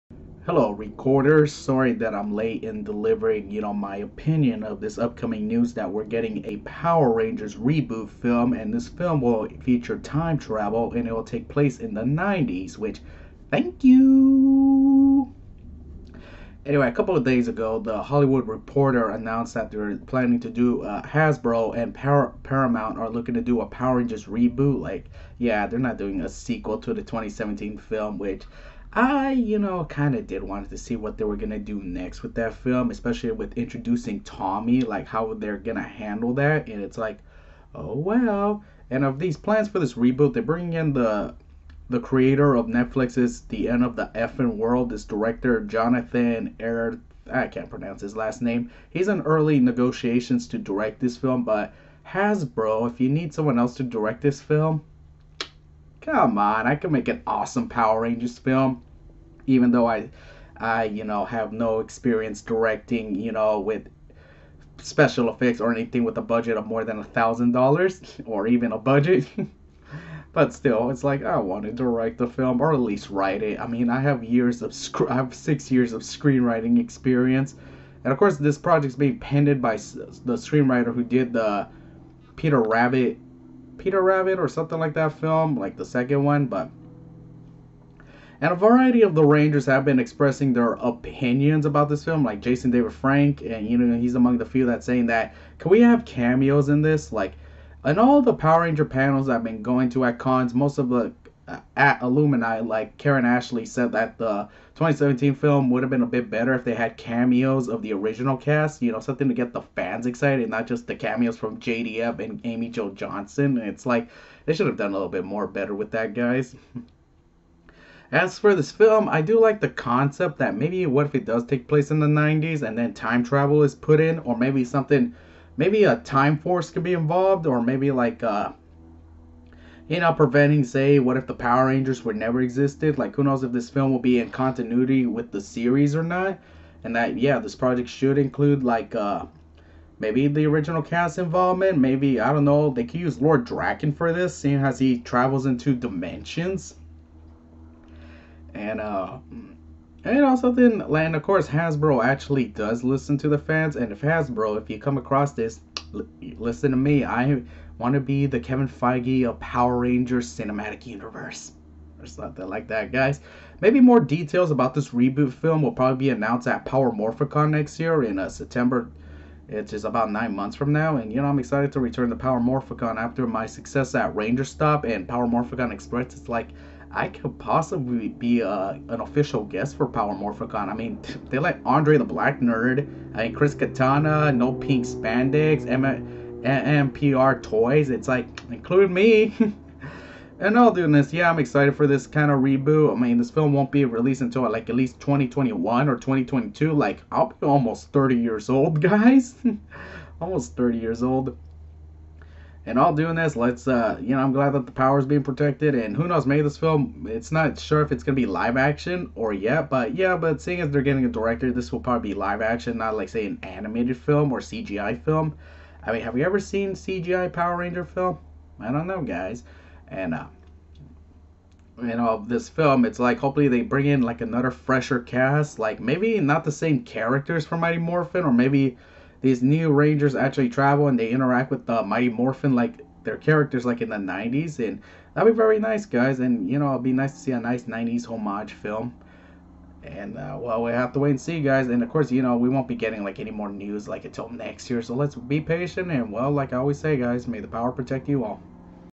Steven's thoughts on Power Rangers Reboot Film
Its Recording Time Media presents.

It’s Morphin time for Jonathan Entwistle

The filmmaker, perhaps best known for creating Netflix series The End of the F---ing World, is taking on Power Rangers, a new version of the colorful family adventure franchise, this time set up at Paramount Pictures.

The following video is the property of
Its Recording Time Media. It is prohibit to copy, download, and reupload any content without permission from Steven Alvarez.